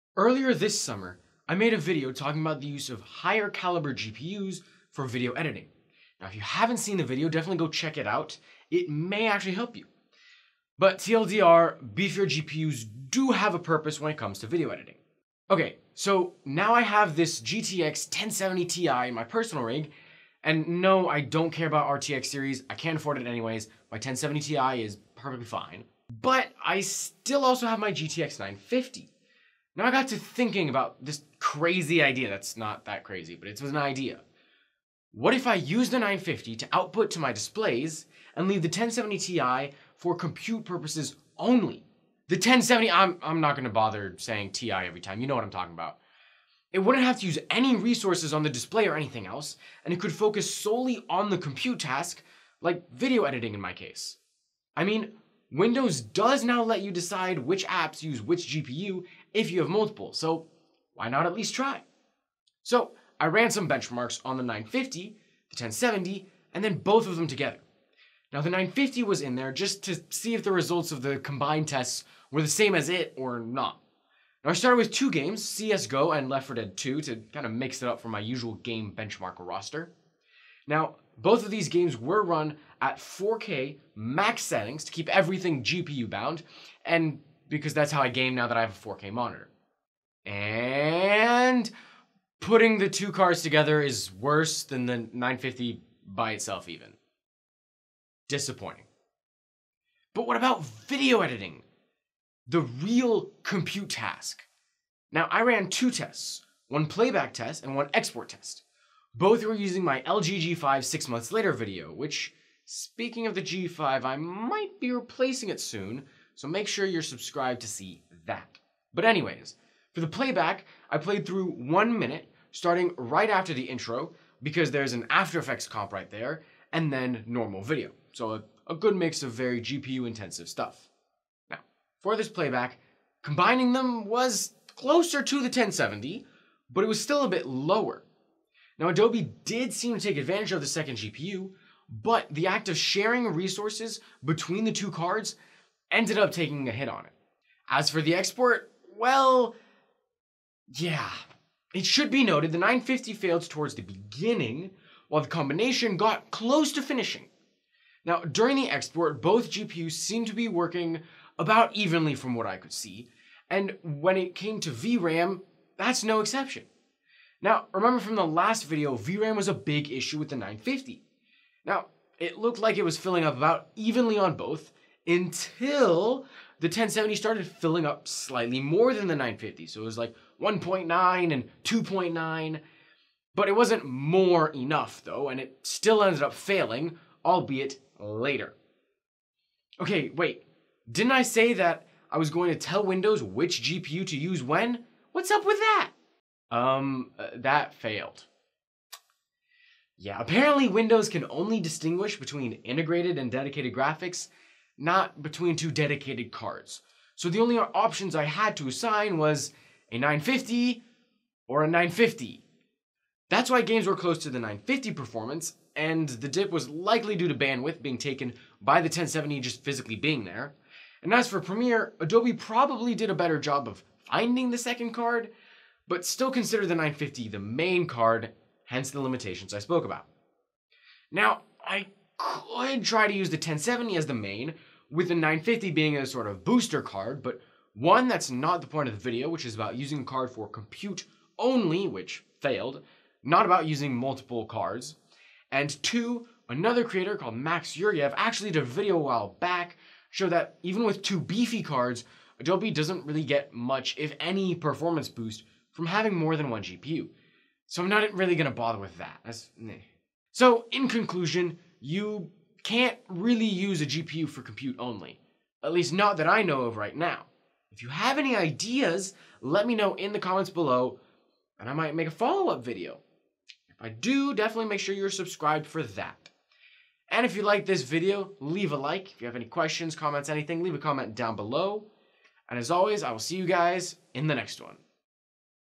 Using a second GPU for compute ONLY?!?
Can you have 1 GPU dedicated to display, and a second used only for compute tasks like video encoding?